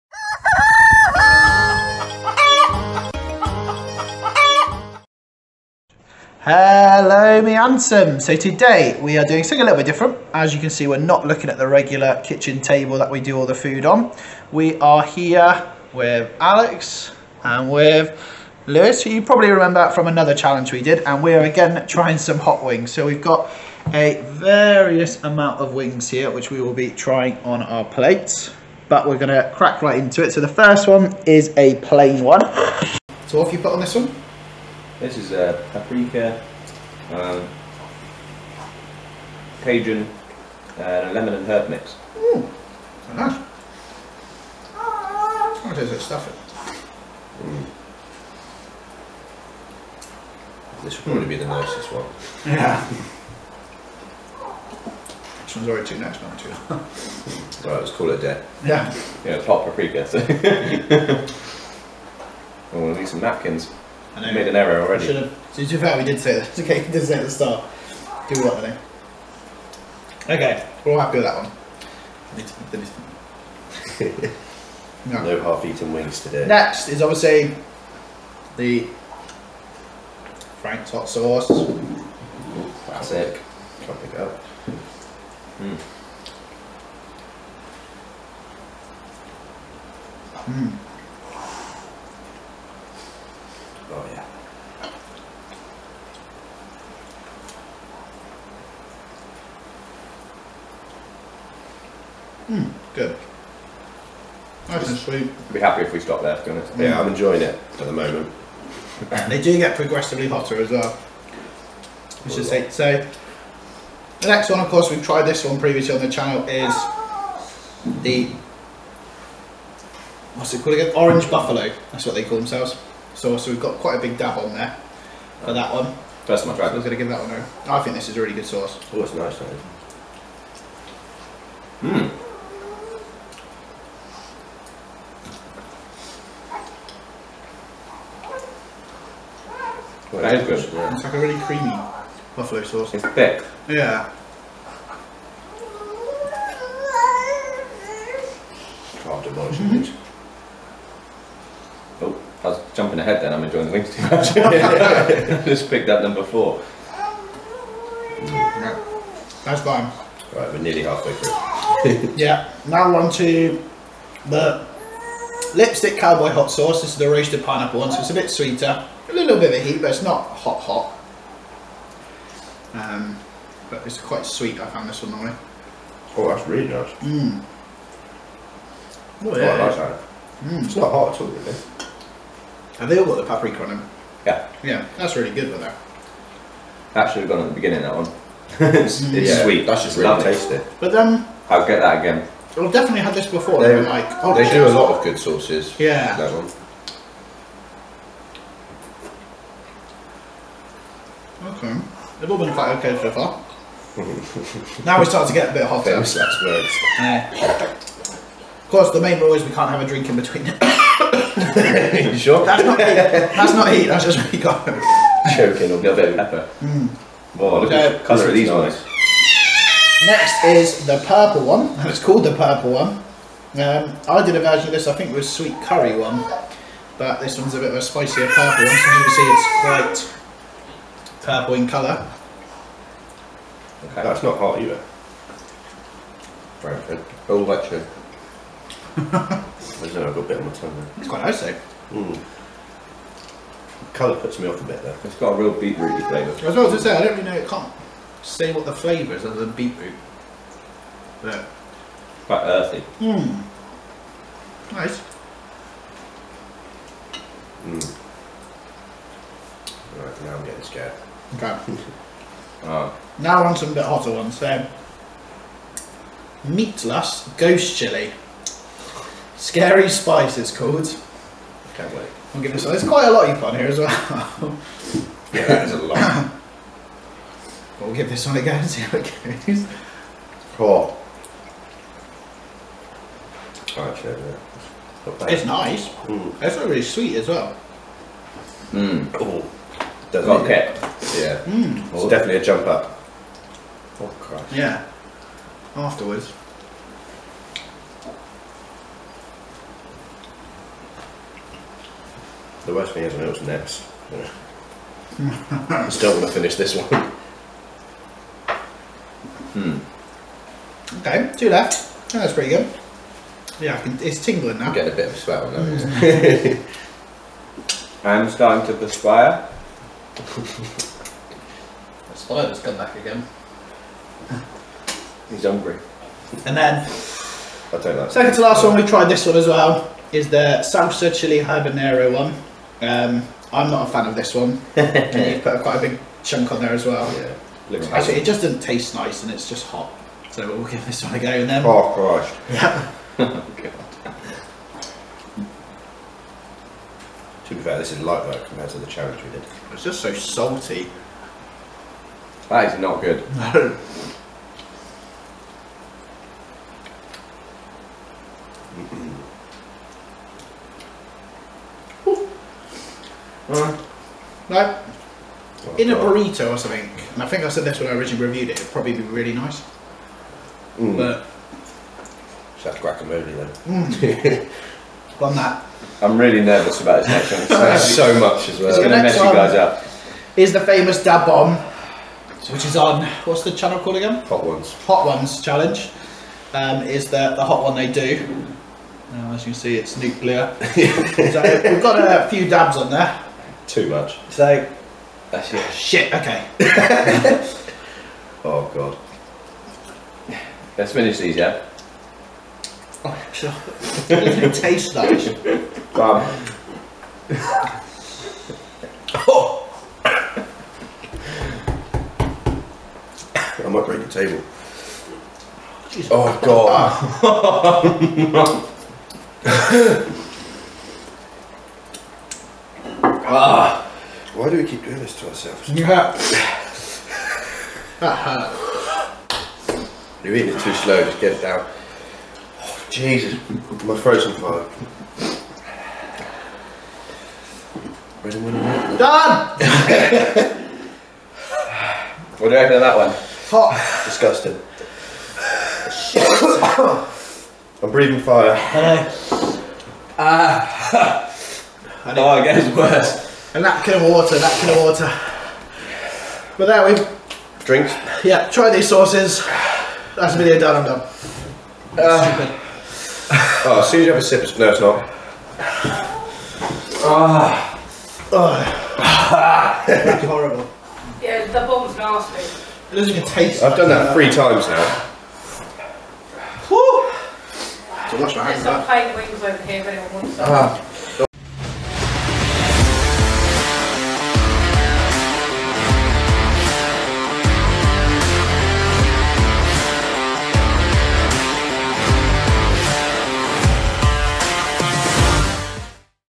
Hot sauce Challenge
This was a mad one!
THis is our attempt at a hot wings challenge with the sauces getting progressivly hotter.

I'm on Instagram as @watchmewingsit. Install the app to follow my photos and videos.

Ta for now!